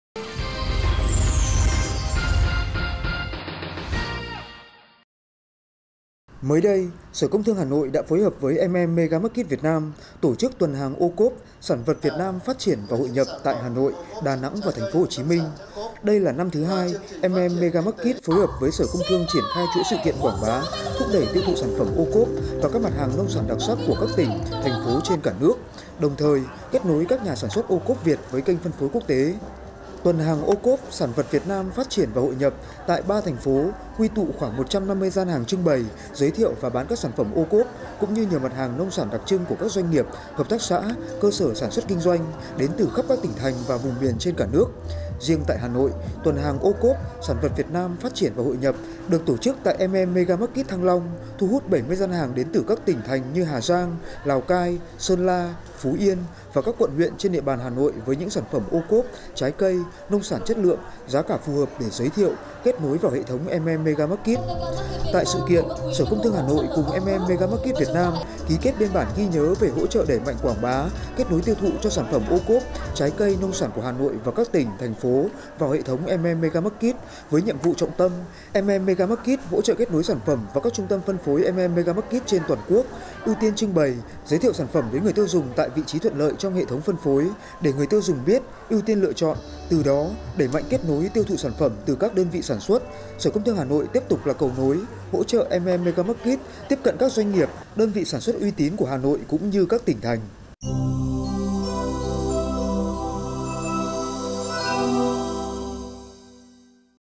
Khai mạc “Tuần hàng OCOP – Sản vật Việt Nam phát triển và hội nhập”
Mới đây, Sở Công Thương Hà Nội đã phối hợp với MM Mega Market Việt Nam tổ chức “Tuần hàng OCOP – Sản vật Việt Nam phát triển và hội nhập” tại Hà Nội, Đà Nẵng, TP. Hồ Chí Minh. Đây là năm thứ 2, MM Mega Market phối hợp với Sở Công Thương triển khai chuỗi sự kiện quảng bá, thúc đẩy tiêu thụ sản phẩm OCOP và các mặt hàng nông sản đặc sắc của các tỉnh, thành phố trên cả nước, đồng thời, kết nối các nhà sản xuất OCOP Việt với kênh phân phối quốc tế…